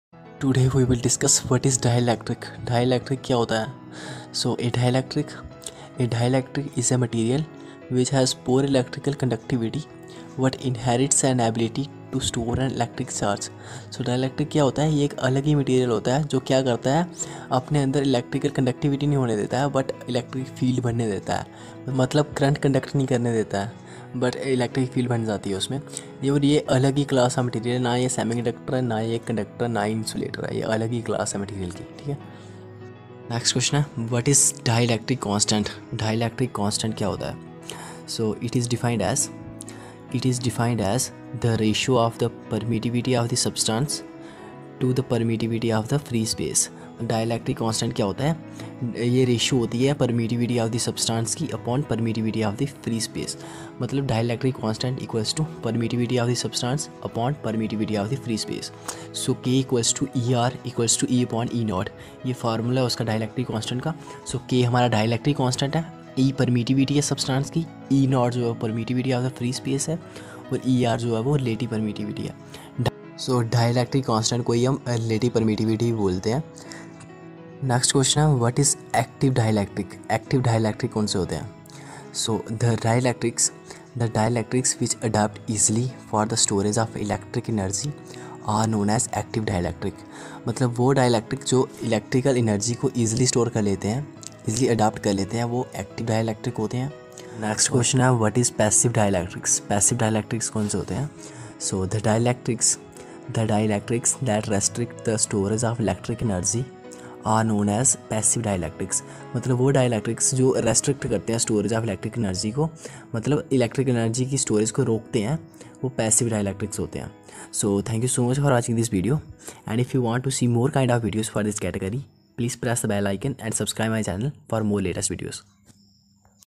Q.What is dielectric ? What is dielectric constant, active dielectrics and passive dielectrics ?
active dielectrics
passive dielectrics